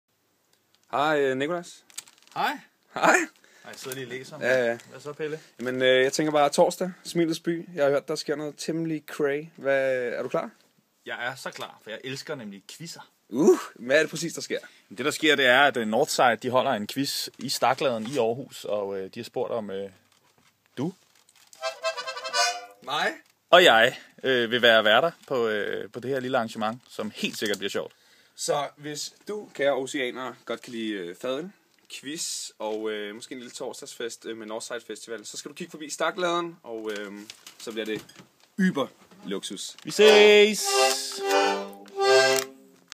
Aarhus Universitet og NorthSide præsenterer Musikquiz
En hilsen fra Pelle Peter og Nicholas Kawamura
Husk at angiv dit holdnavn

Hvad kan man vinde?
Til vinderholdet: billetter til NS13
Tredjepræmie: Rejseguides fra Aarhus Universitetsforlag
Til de der har fortjent det: Overraskelser undervejs fra Royal Beer
Vil du vide mere — her: Stakladen.